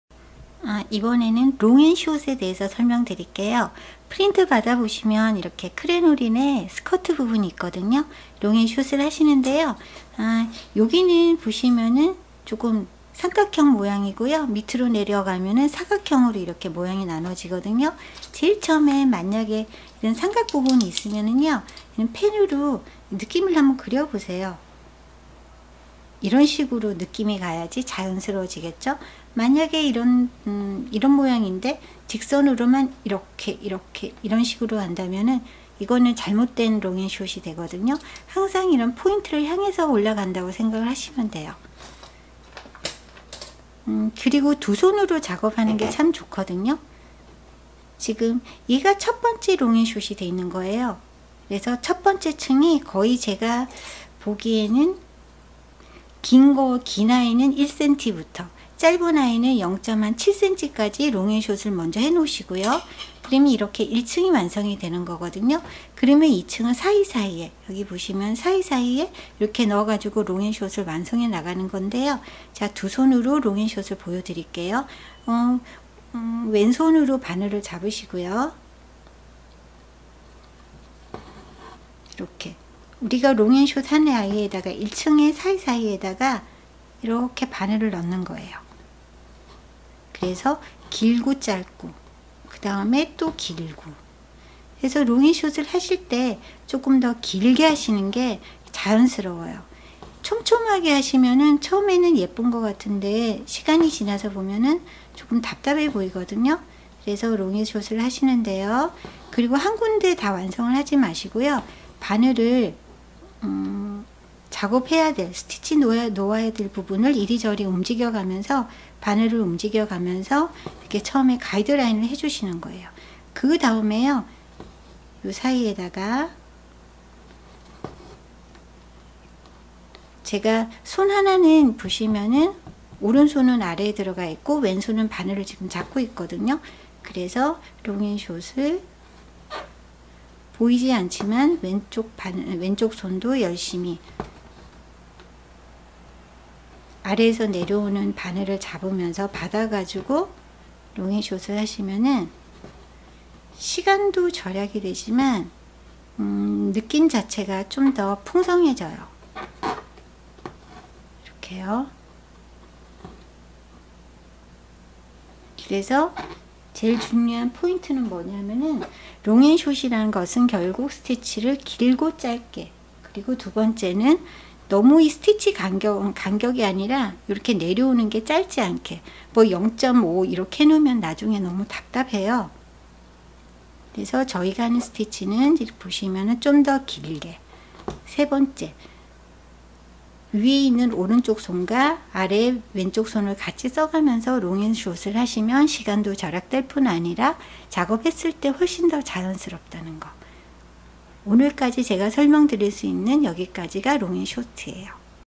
자수방법설명해요 long&short stitch
넒은면을 메꾸는 자수방법을 설명해요
It show how did i stitched long&short in my embroidery project:)